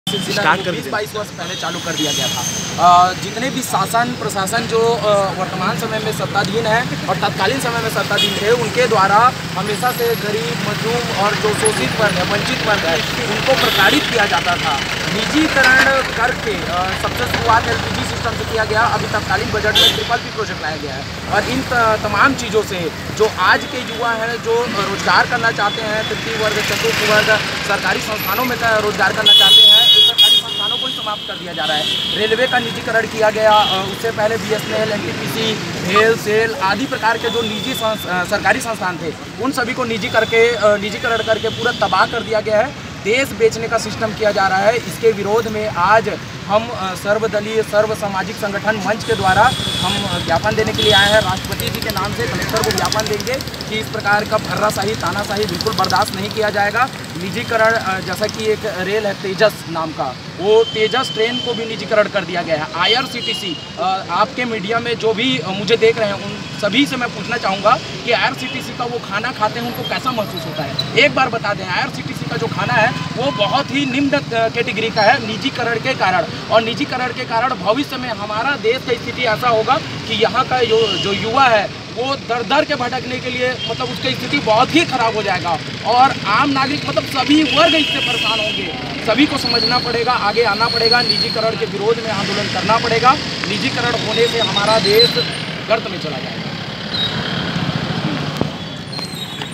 निजीकरण के विरोध मे सर्व दल एवं सर्व समाज के लोगो ने रैली निकालकर कलेक्टर को सौंपा ज्ञापन
रायगढ़. भारत के शासकीय संस्थानों व शासकीय कंपनियों के निजीकरण/निगमीकरण व ठेकेदारी प्रथा के खिलाफ शनिवार को रायगढ़ में विरोध प्रदर्शन किया गया।